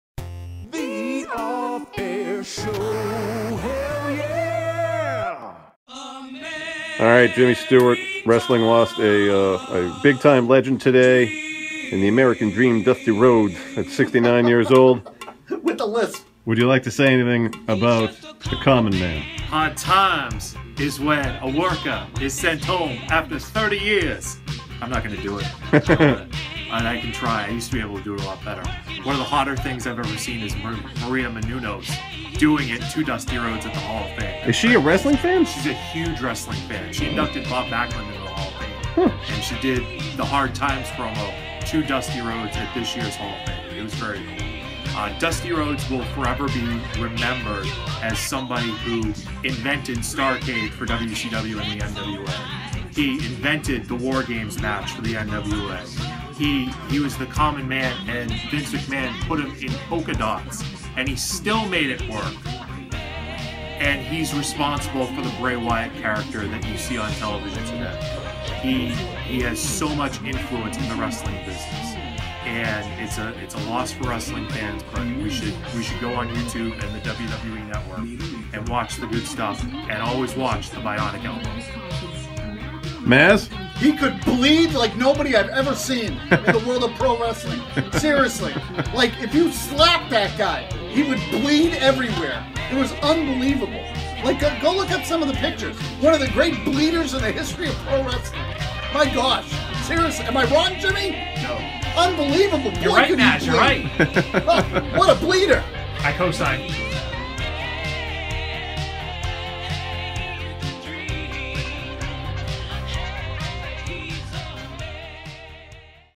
OffAirShow 38
J-Stew gives a eulogy for the legendary Dusty Rhodes